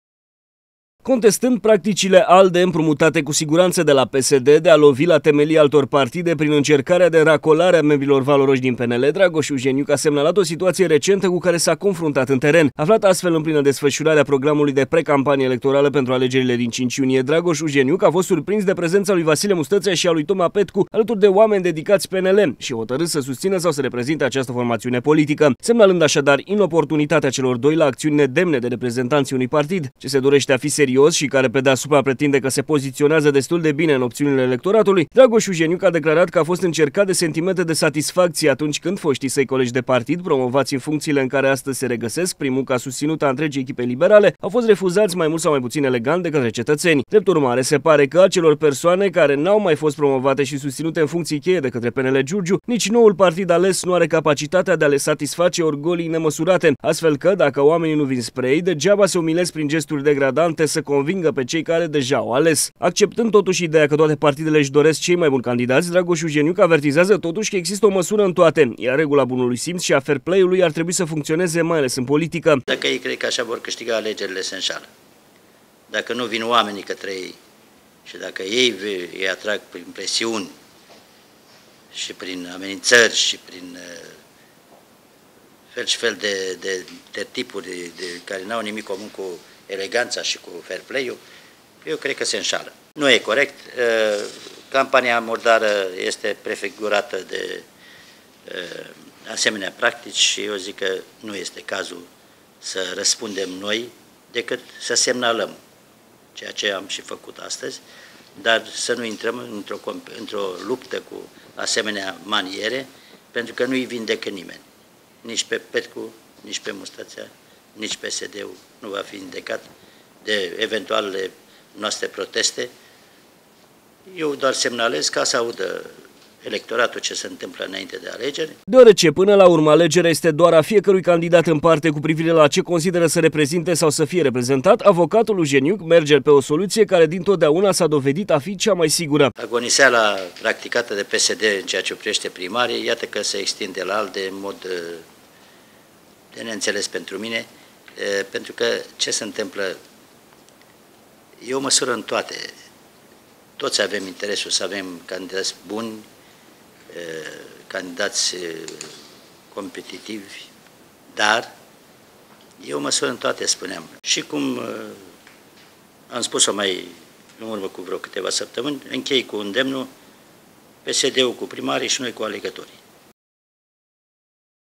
ALĂTURAŢI DE ORGOLII POLITICE
ALEGÂND ALT PARTID PENTRU SATISFACEREA ORGOLIILOR POLITICE, VASILE MUSTĂŢEA ŞI TOMA PETCU NU SE REGĂSESC, SE PARE, NICI PRINTRE FAVORIŢII ALDE.AFLÂNDU-SE, ASTFEL, PRINTRE MARII PERDANŢI AI POLITICII LIBERALE, CEI DOI SE CONFRUNTĂ CU O CRIZĂ ACUTĂ DE ELECTORAT